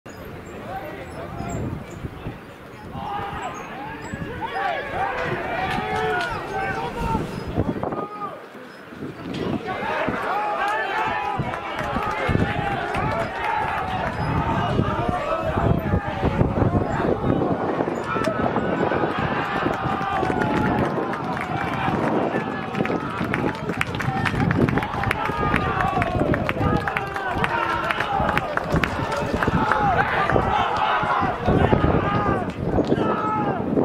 Wie einst Gareth Bale: Irrer Sprint in der Regionalliga Bayern
Was für ein unglaublicher Antritt. Jens Castrop, Nachwuchstalent des 1. FC Nürnberg, legt im Regionalliga-Spiel gegen den 1. FC Schweinfurt 05 einen Sprint über den gesamten Platz hin und krönt seine Energieleistung mit dem Treffer zum 2:0. Was für eine geile Szene!